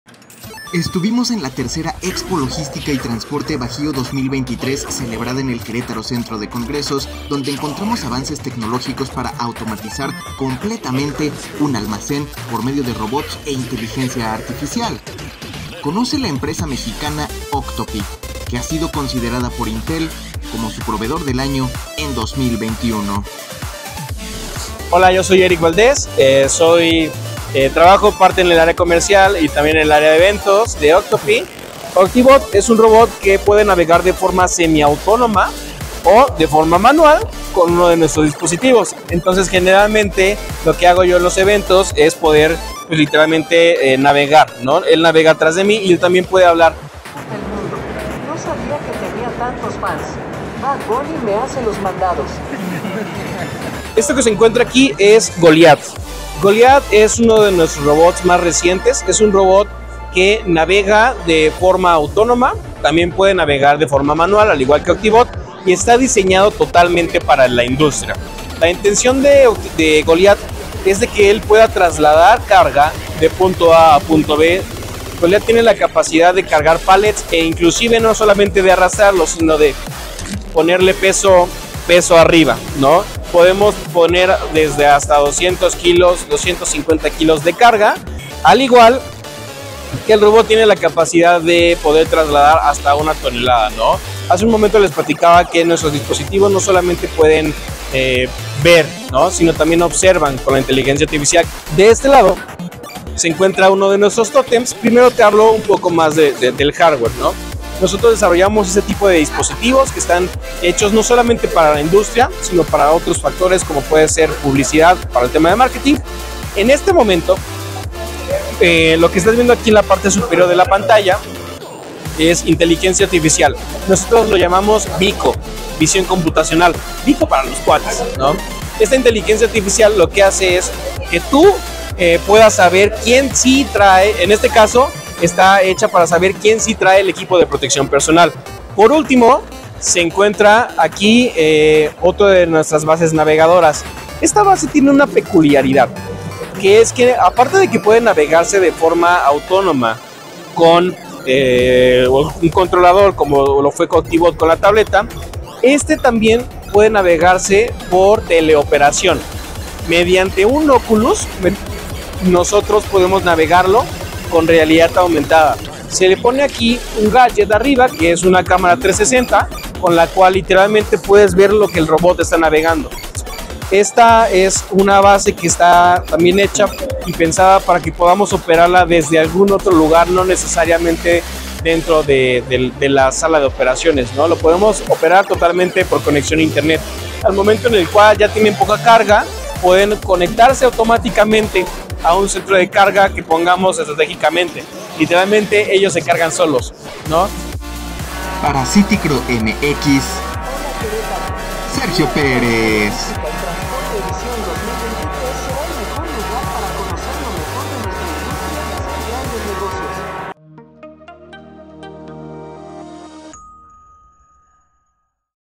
Con esta tecnologia ¡un almacén puede ser operado automáticamente!
Estos robots pueden manejar pallets y cargas de hasta 1 tonelada, y monitorear las 24 horas ¡hasta quien trae el equipo de seguridad!. Conocimos la empresa mexicana Octopy en la 3ra Expo Logística y Transporte bajío 2023, realizada en el queretaro Centro de Congresos.